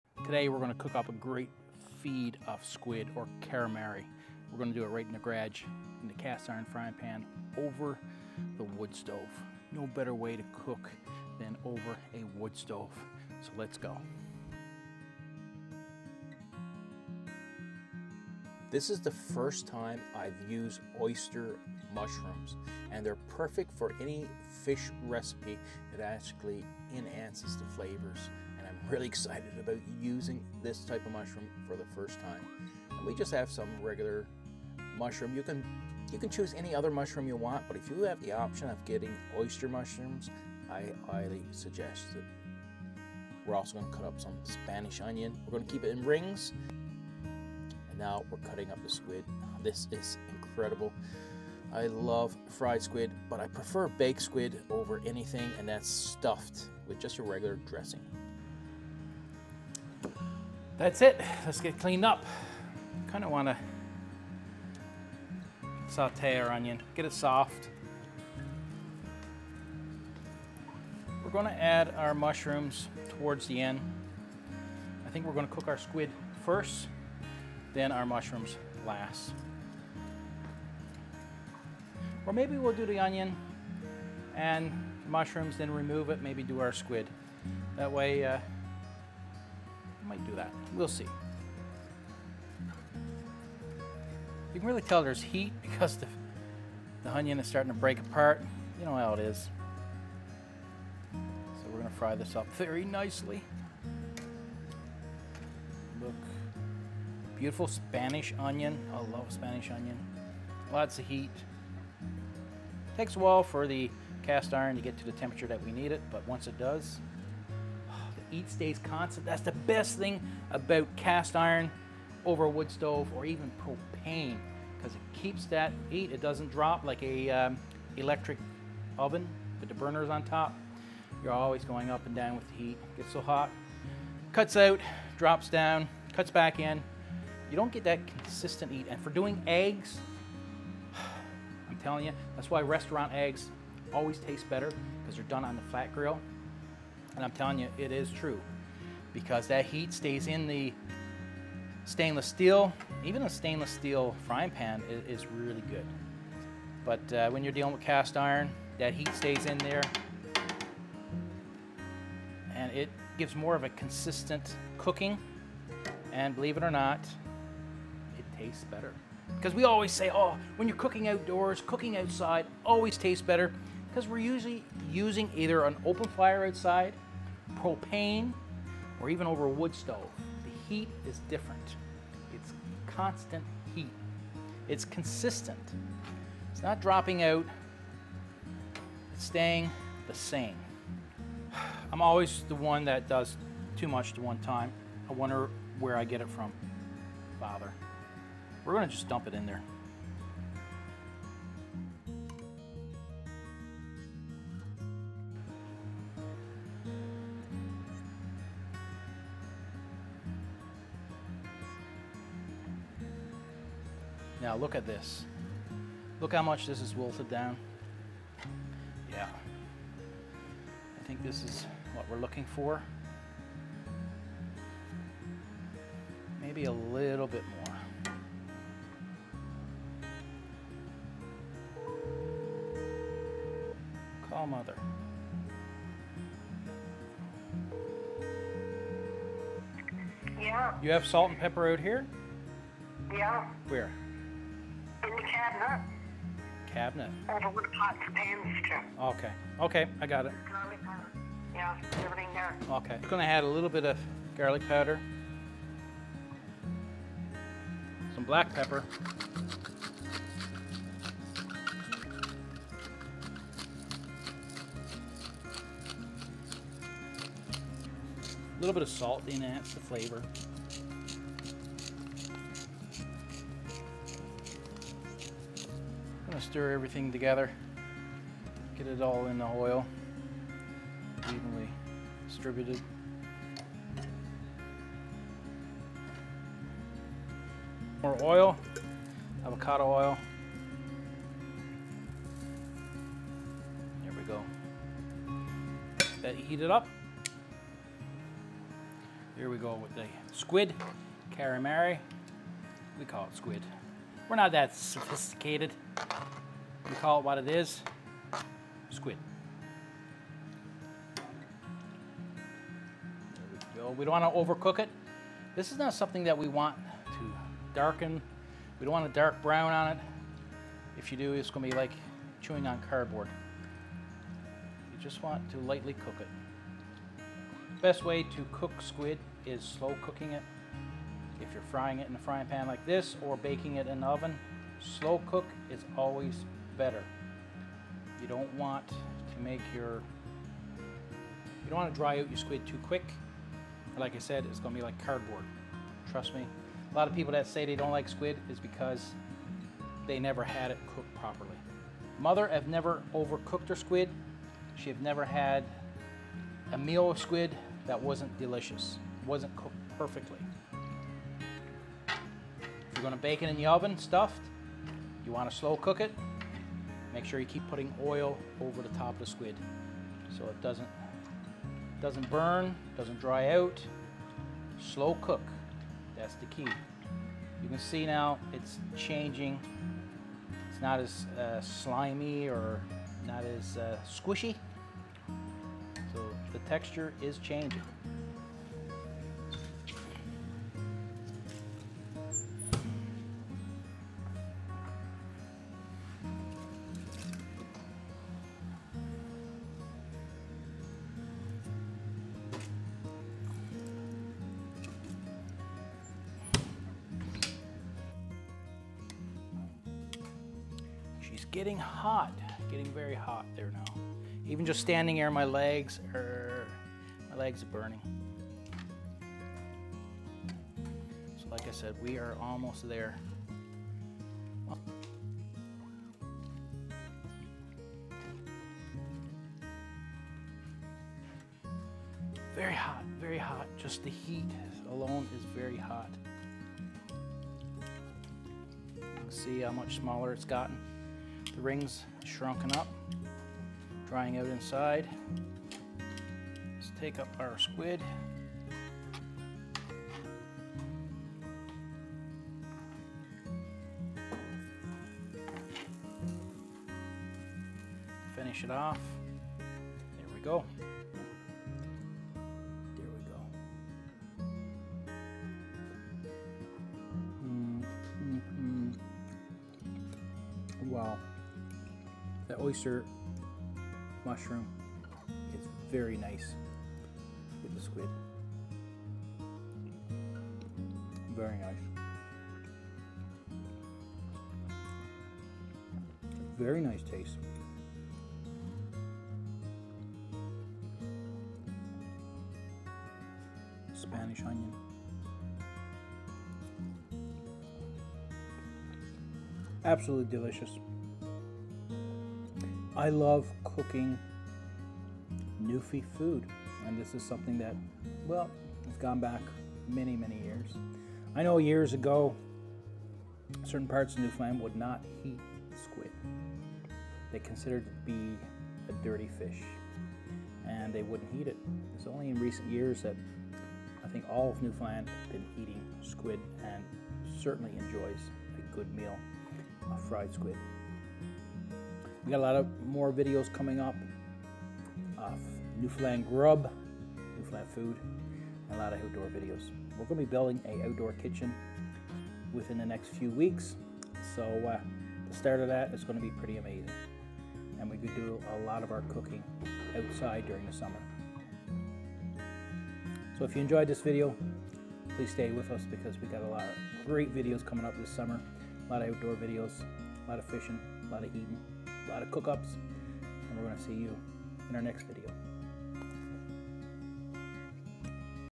Cooking Up Some Fried Squid
We're diving into the world of cast iron cooking over a wood stove to create a delicious dish of fried squid.

This is where I buy All my Cast Iron Cookware.

My FaceBook Group Please Join!

Check out their site for free music.